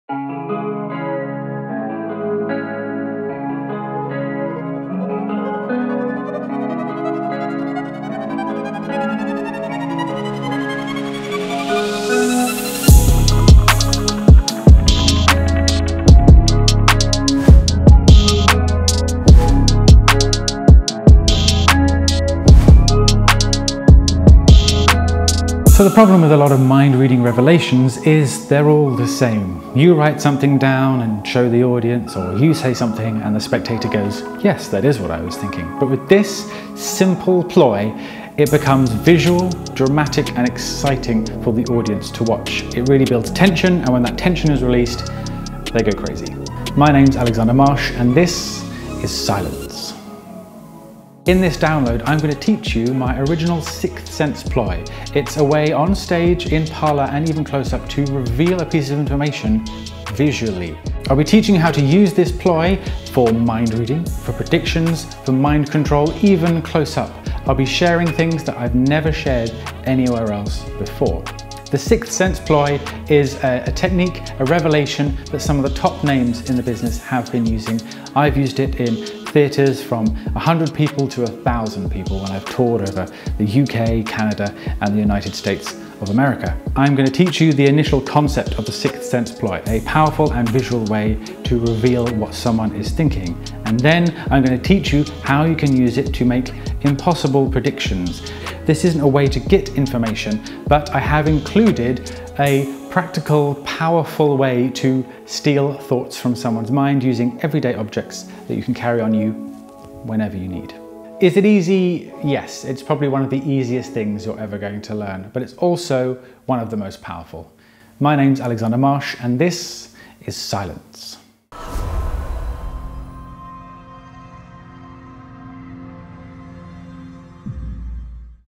Silence by Alexander Marsh || A Propless Ploy You Can Add to Your Stage and Close Up Mentalism
SIGN UP FOR OUR NEWSLETTER AND LEARN 10 FREE MAGIC TRICKS

With Silence by Alexander Marsh, you learn a process that you can apply to stage, close up, and propless performances.  It makes your mentalism play BIG, and creates a HUGE amount of drama.

Tell a stranger their star sign, without having them say anything out loud, or write anything down.

Appear to take control of the participant's body as they perform a series of random motions.

"A fantastic way to make your reveals visual!" - Luca Volpe

This method allows you to BE BOLD.

Make an impossible guess about ANY thought a spectator might have. If you get it right, you have pulled off a miracle! If you get it wrong you create a theatrically interesting moment of planned tension.